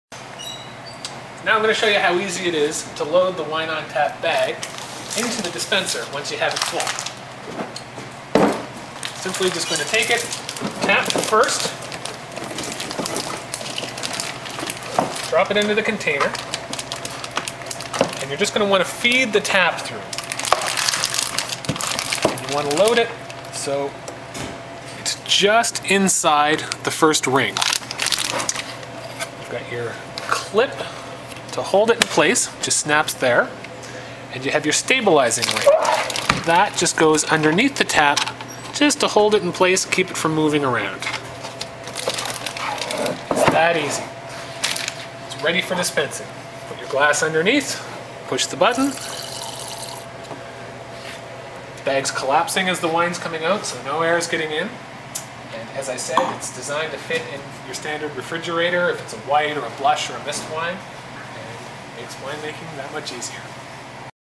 Noble Grape Wine Making Kits: Wine-on-tap dispensing
| Using a Wine-on-tap to dispense your wine. Learn how to make your own wine with Noble Grape.